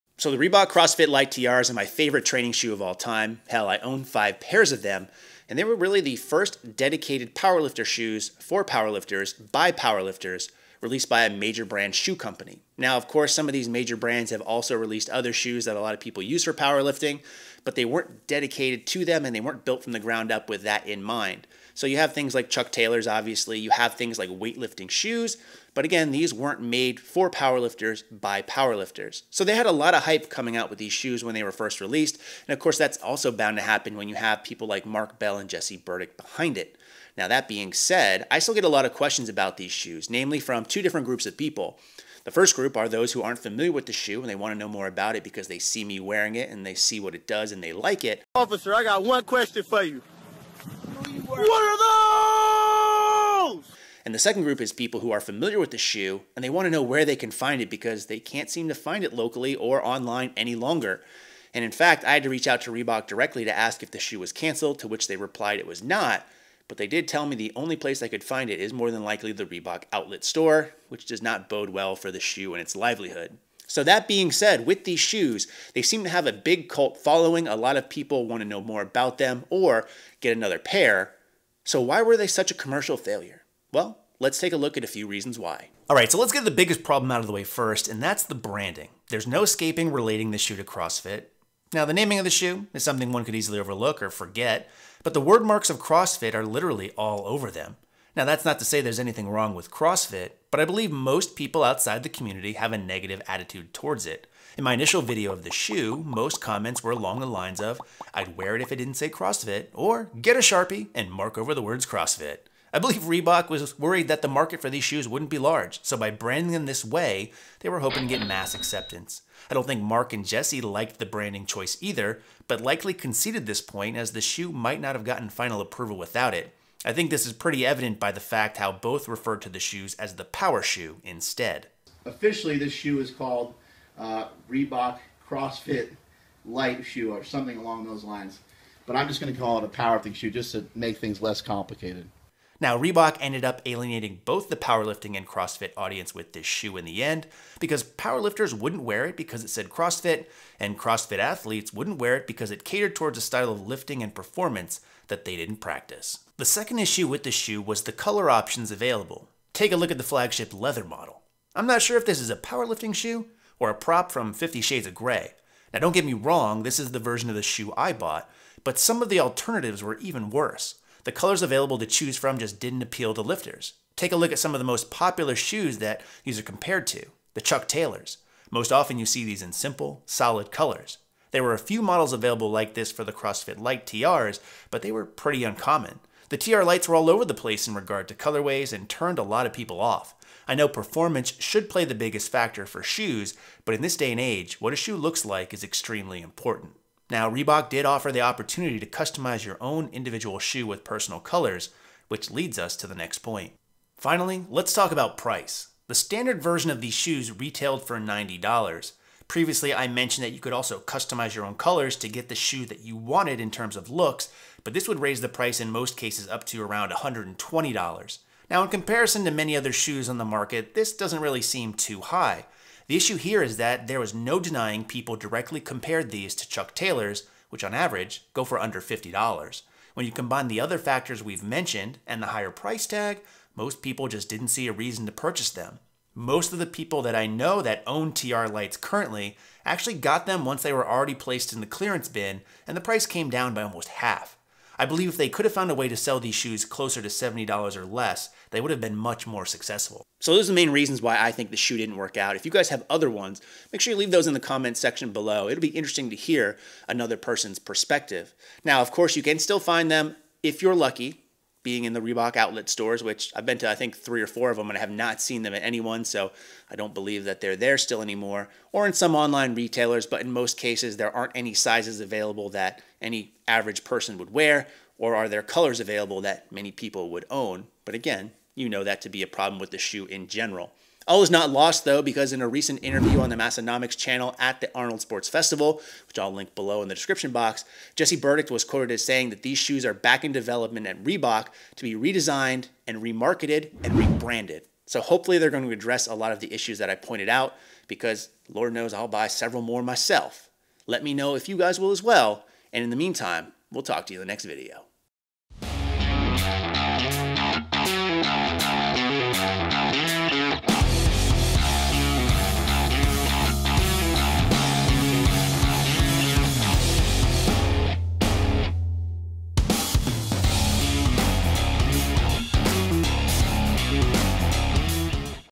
Why the Reebok Crossfit Lite TR Failed
The Reebok Crossfit Lite TRs are my favorite training shoe of all time. They seem to have amassed a cult following in recent months as they're extremely hard to find now. In this video I talk about why they failed, and what we can expect in the future.